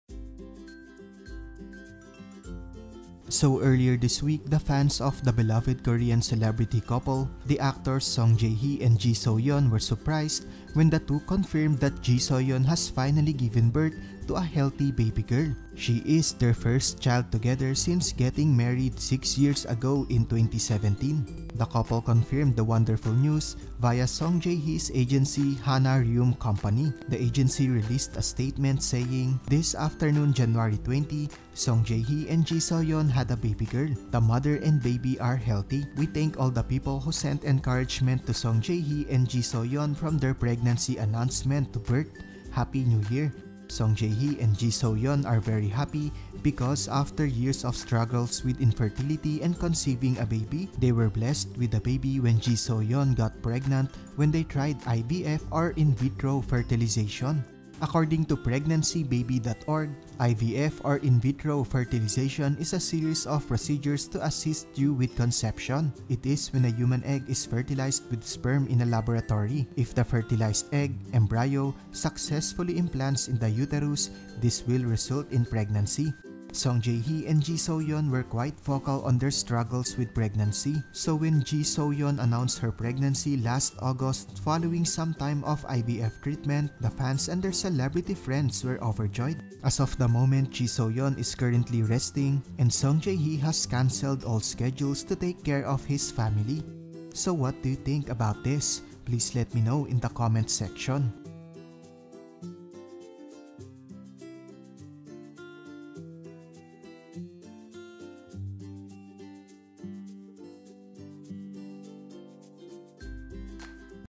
Song Jae Hee is Now a Dad after his Wife Gives Birth to a Baby Girl! 🥺
The popular korean actor Song Jae Hee is now finally a father, after his actress wife Ji So Yeon has given birth to a cute and healthy baby girl. This is after their hard struggles with infertility and conceiving a baby for years since they got married in 2017.